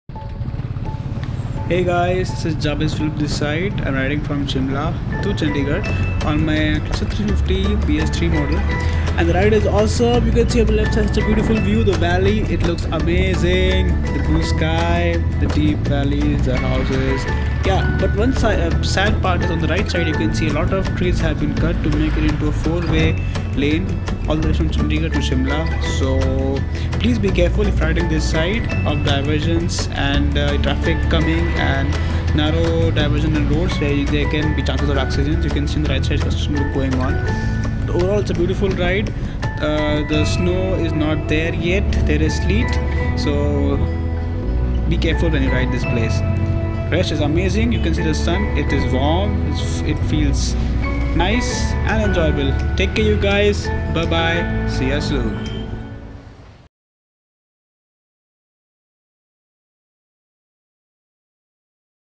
Shimla to Chandigarh
Riding my 350 back to Chandigarh